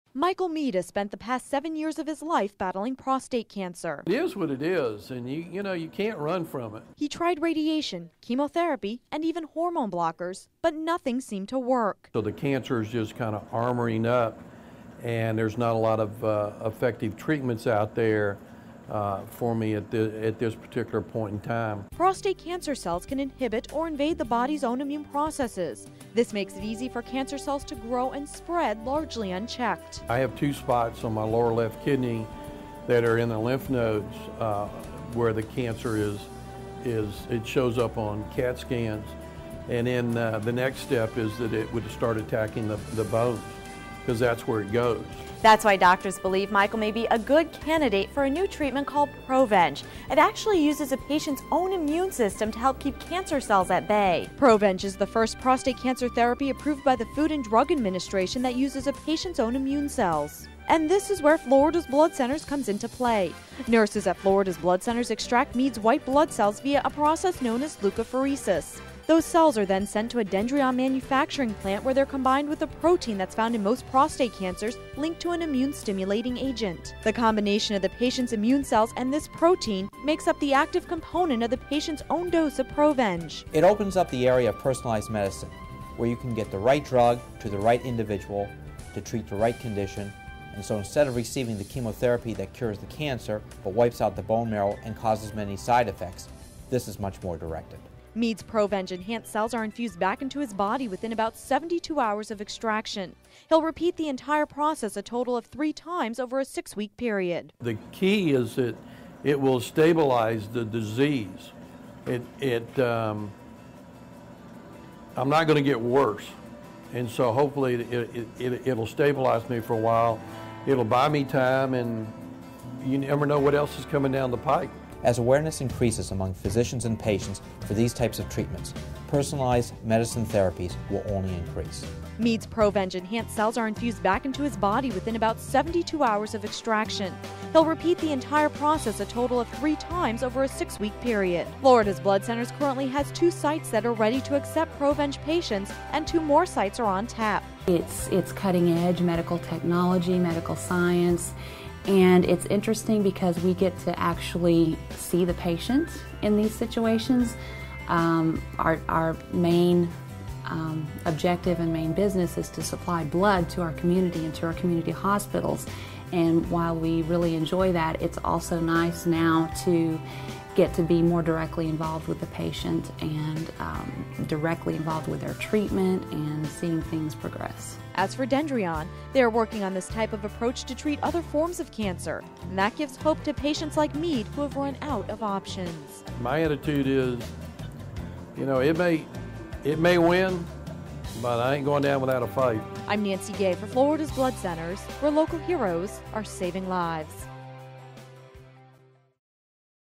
Man Fights Prostate Cancer Using Own White Blood Cells
A new prostate cancer therapy gives prostate cancer patients the opportunity to fight end-stage prostate cancer using their own white blood cells.